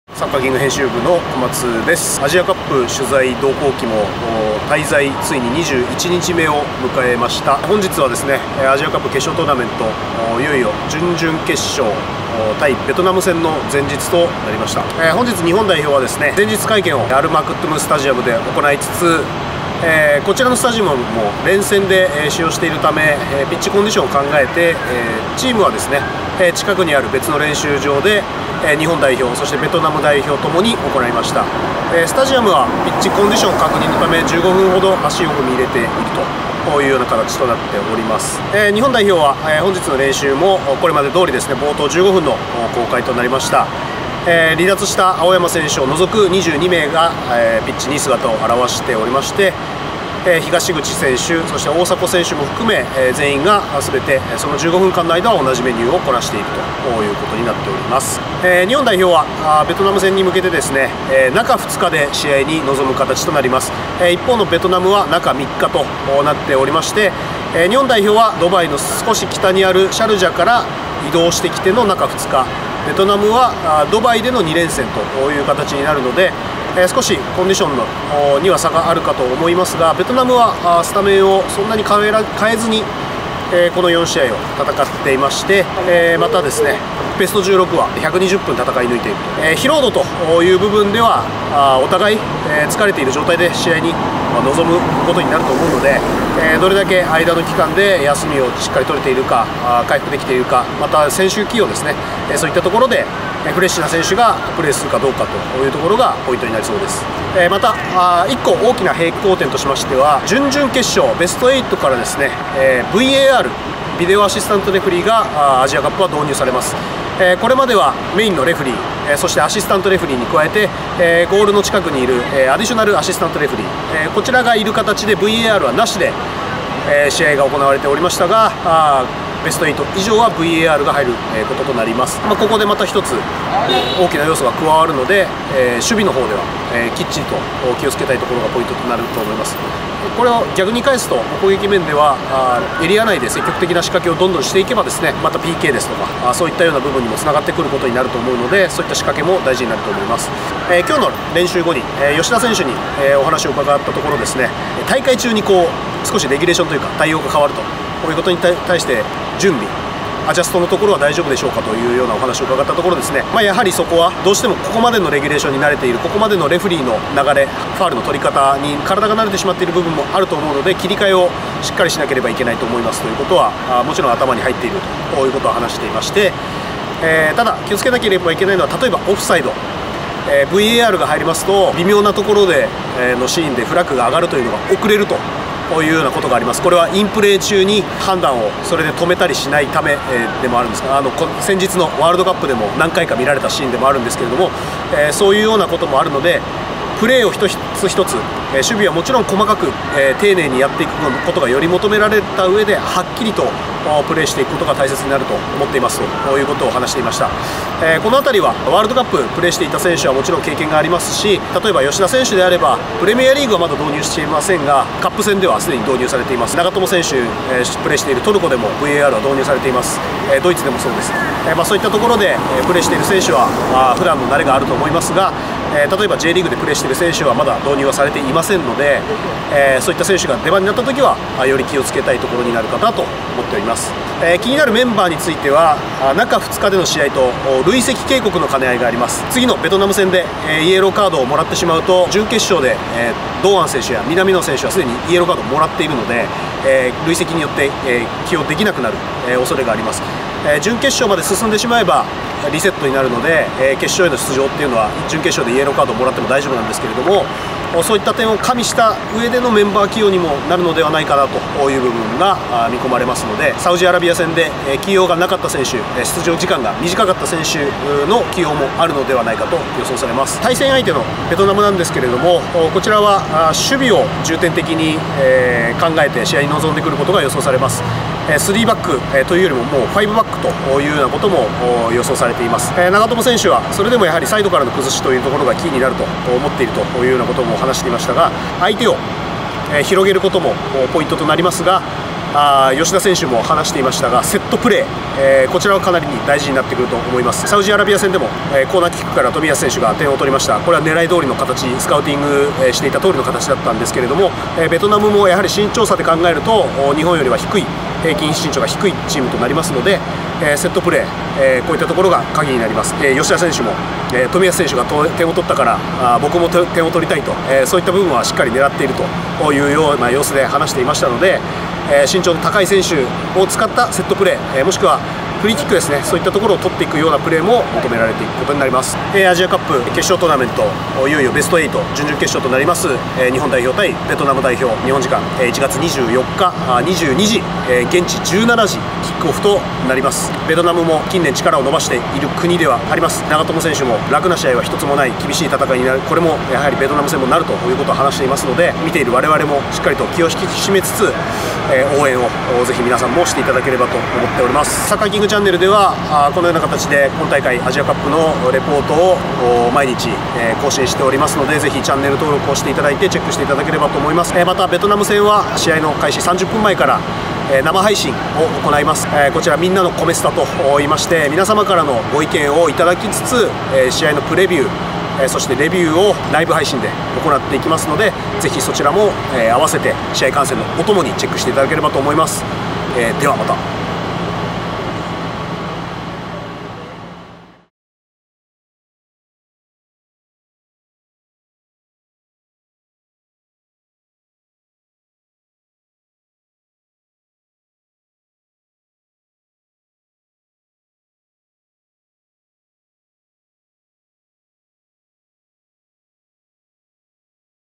【日本代表】ベスト4かけて中2日でベトナム戦【直前レポート】
準々決勝・ベトナム戦の前日となった23日、日本代表はUAE・ドバイで前日練習を実施しました。

現地UAEで取材中の小松春生が、選手コメントを交えたレポートをお届けします。